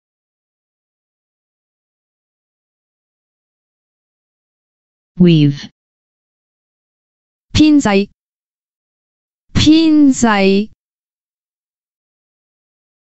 "WEAVE" in Cantonese (編製) - Flashcard
[pin1 jai3] "WEAVE" in Cantonese (編製) - Flashcard. Additional information about the word: weave,plait,braid,work out,draw up,authorized strength,establishment.